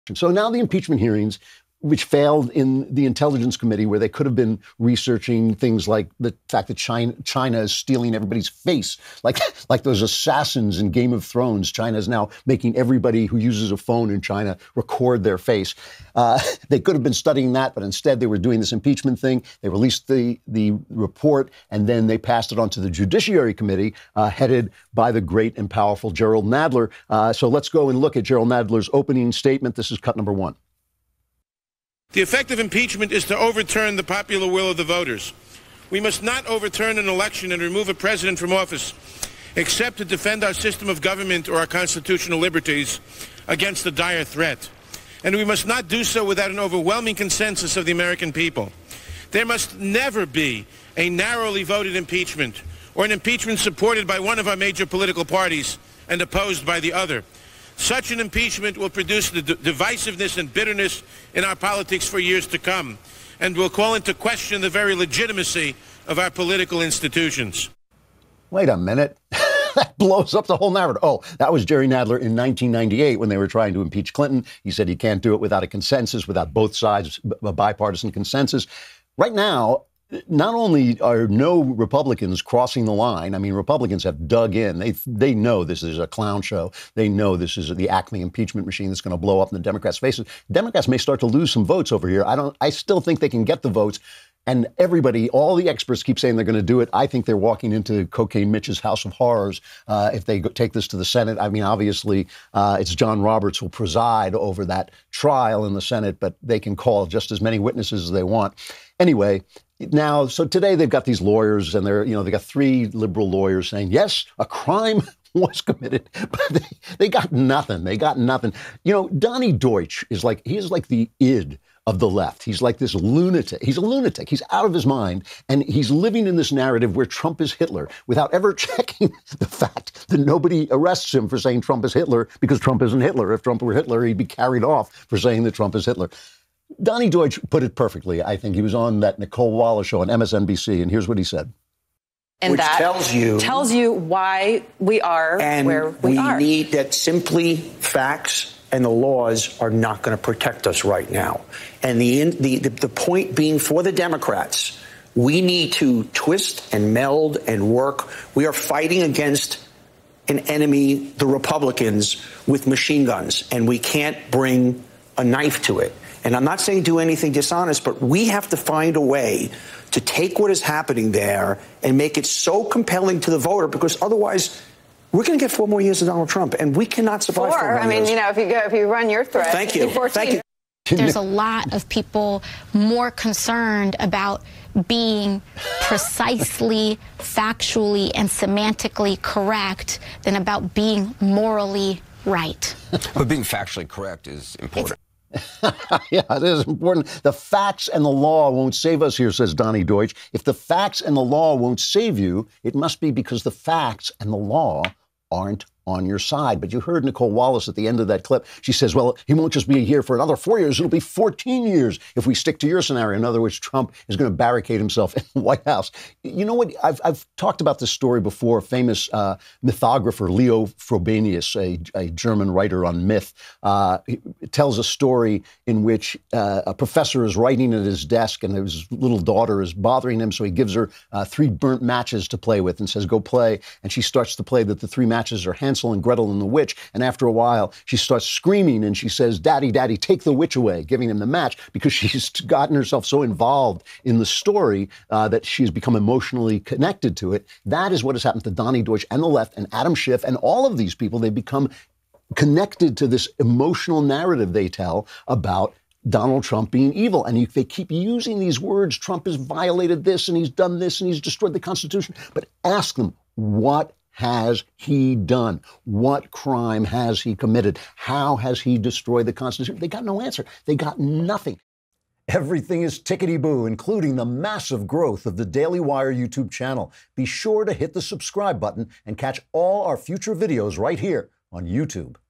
Democrats Accidentally Expose Their GLARING Hypocrisy on Impeachment (Clinton vs Trump)
Andrew Klavan gives the latest update on the impeachment hearings after Jerry Nadler begins a new one in the Judiciary Committee.